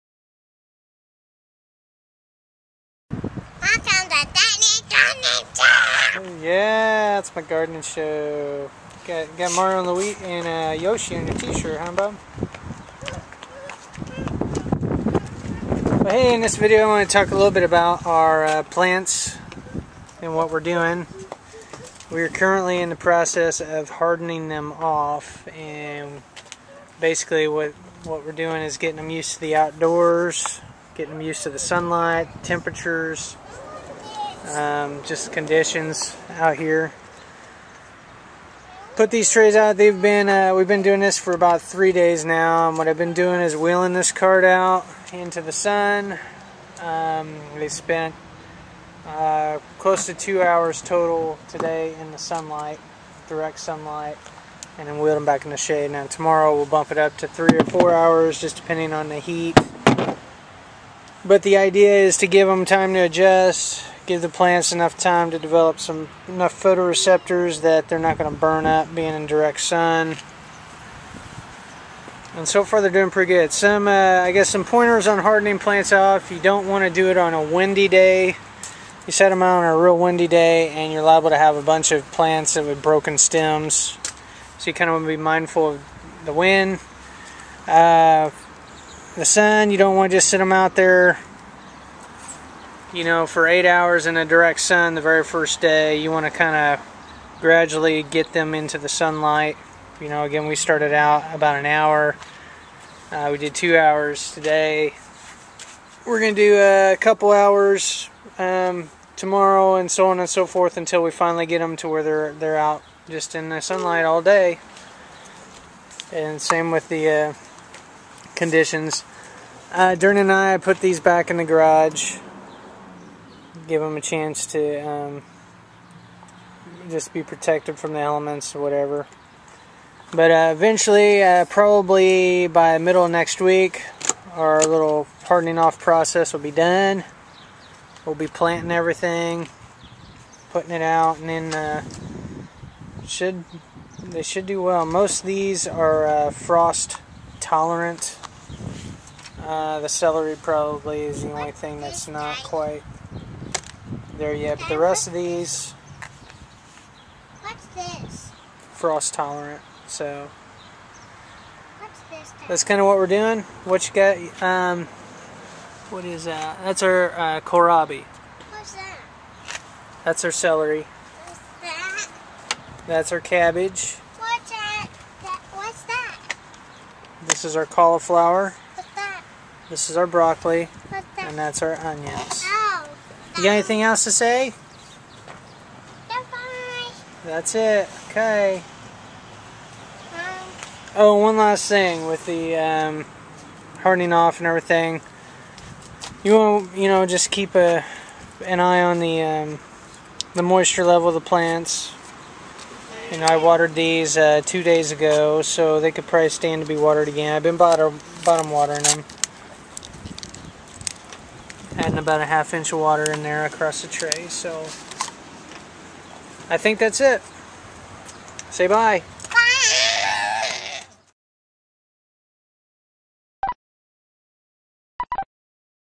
Before You Plant, Harden Off Those Beautiful Plants!
"Hardening off" your seedlings is the process of allowing your plants to get acclimated to the great outdoors.  You'll want to start them out in the shade or cloudy day and then gradually introduce them to full sunlight.  On the next day, put them out in the sun for 1 to 2 hours, then move them back to the shade.  Repeat the process each day, increasing your plants' exposure to full sun by an hour each day.  By the end of the week they'll be adjusted to the outdoors, but take your time.  It's better to be safe than deal with sun scald and withered plants.   In fact, I usually give them 2 weeks to harden off, bumping up their exposure to light every other day, instead of every day.  Take your time, and your patience will be rewarded.